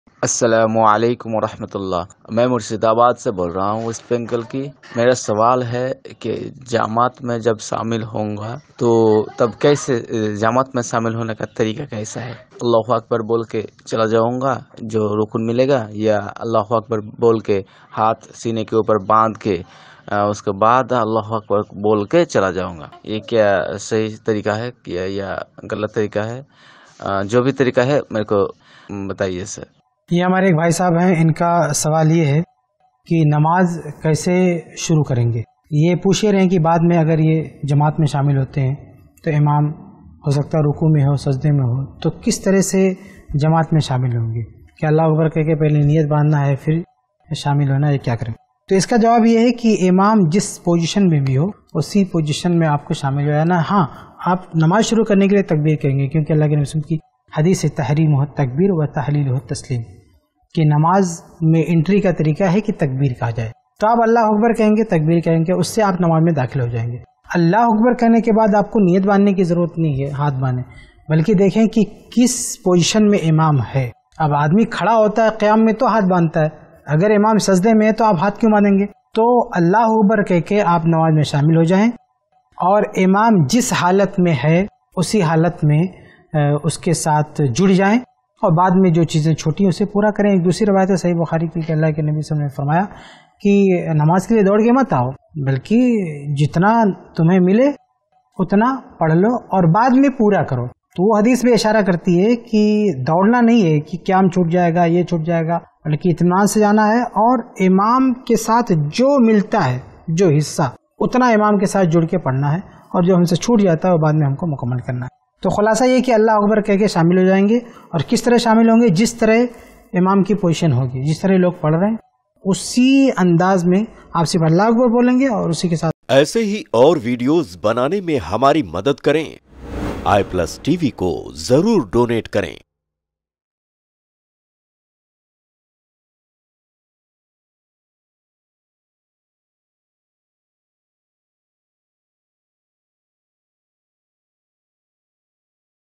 Agar Imam Ruku Sajde Mein Ho Toh Namaz Mein Kaise Shamil Honge? | Shaikh Kifayatullah Sanabili
Agar Imam Ruku Sajde Mein Ho Toh Namaz Mein Kaise Shamil Honge? | Shaikh Kifayatullah Sanabili | iPlus TV